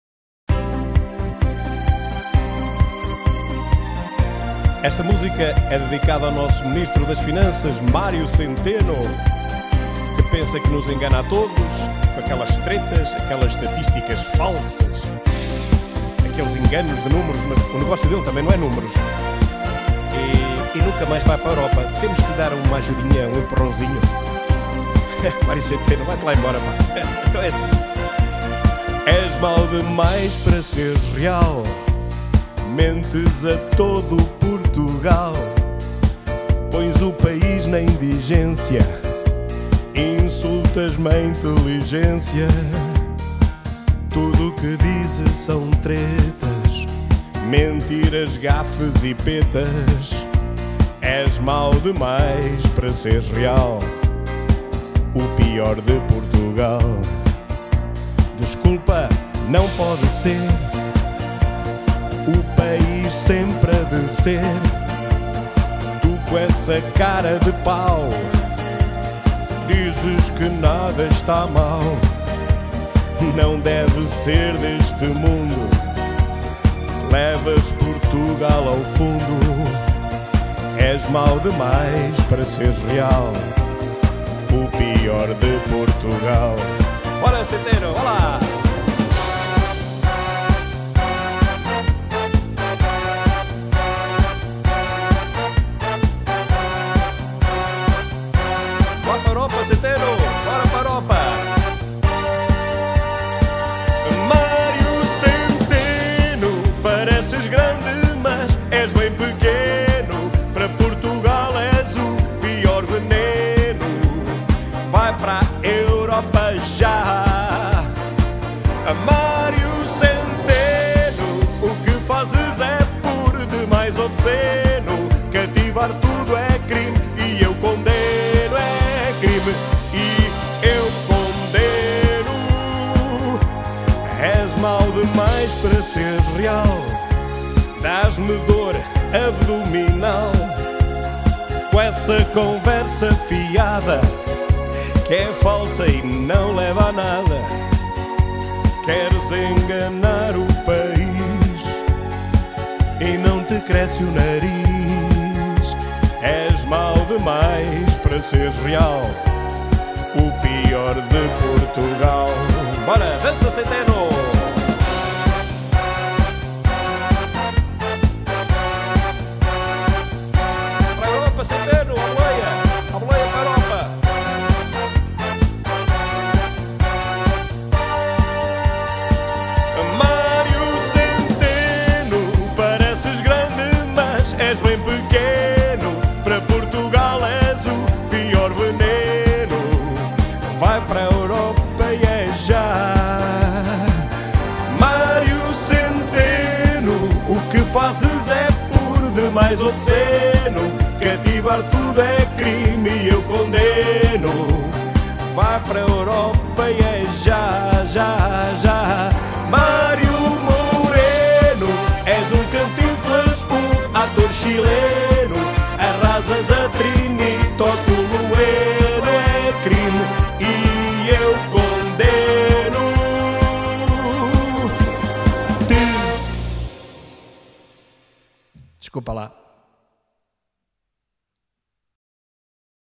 Mário Centeno - o pior de Portugal
Na comemoração dos 10º aniversário do meu mural no facebook aqui vai um tributo ao tio Mário. Moreno.